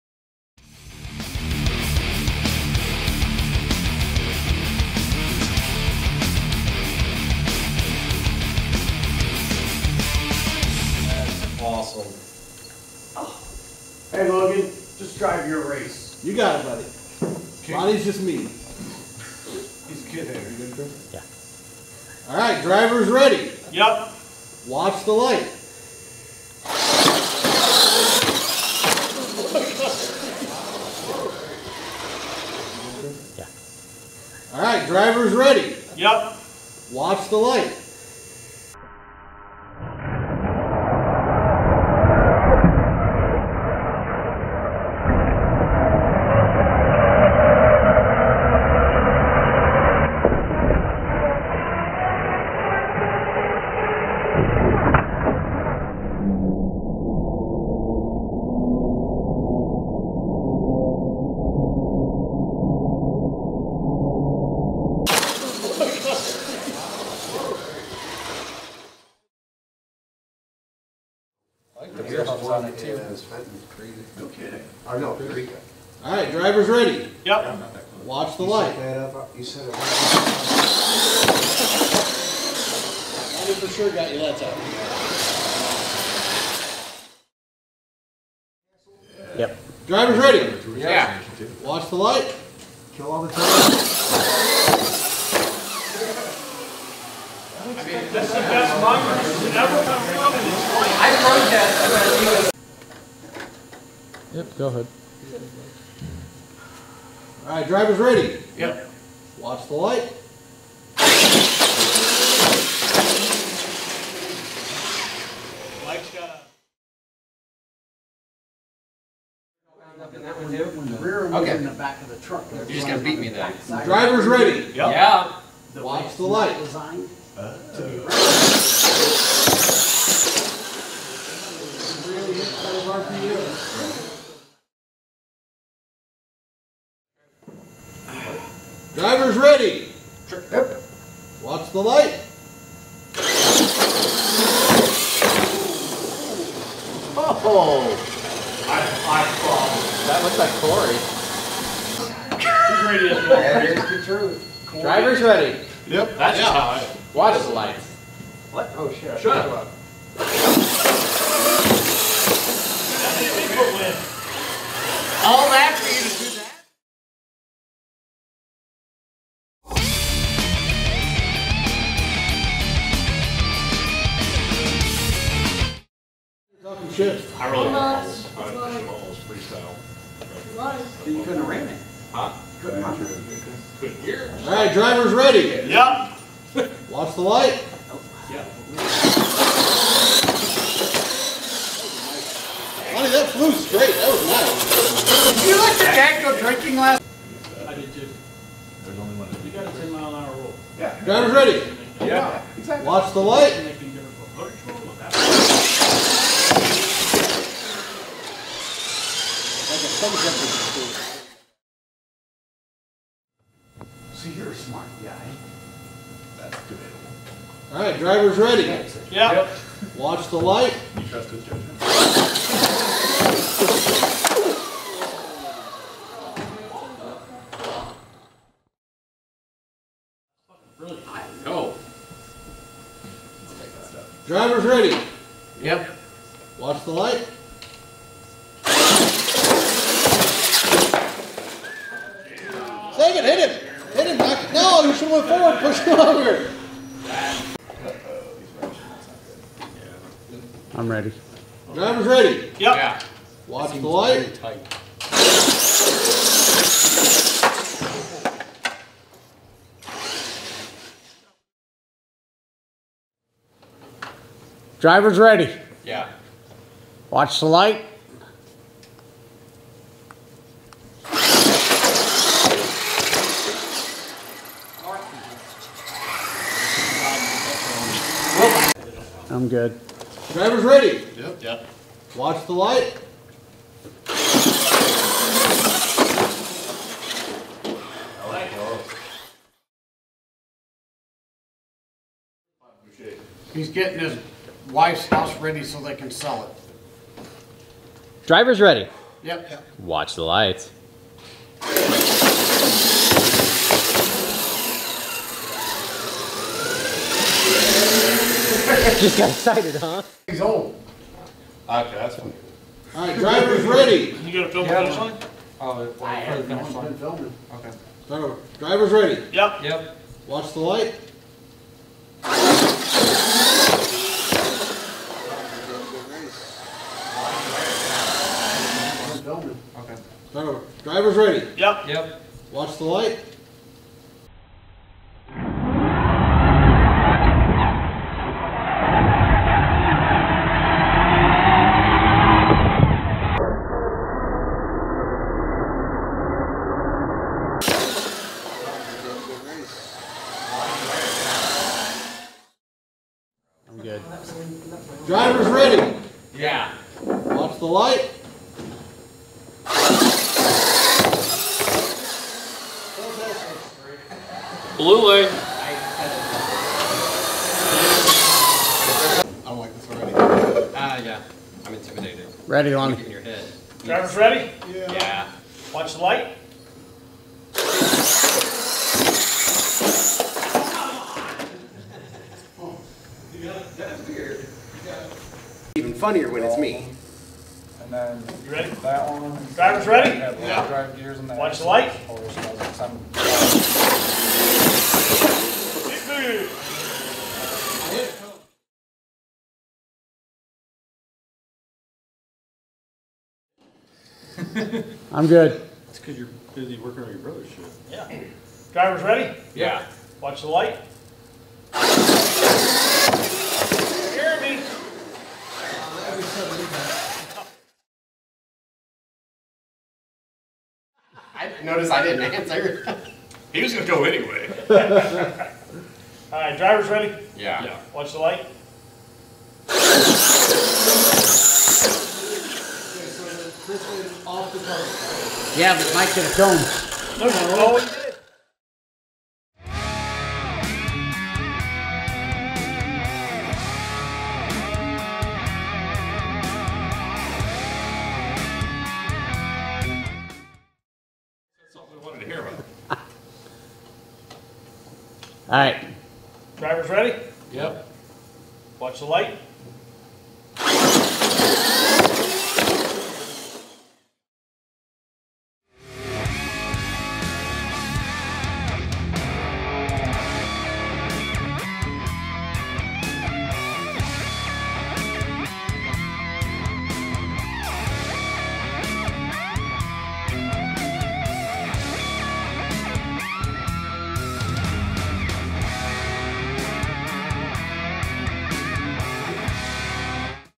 Sport Mod Thunder Drags! Feb.5, 2023 Childress R/C Monster Trucks
Sport Mod Thunder Drags, just like how Penda used to run back in the 90's.  Childress R/C Racing provides the action from February 5, 2023.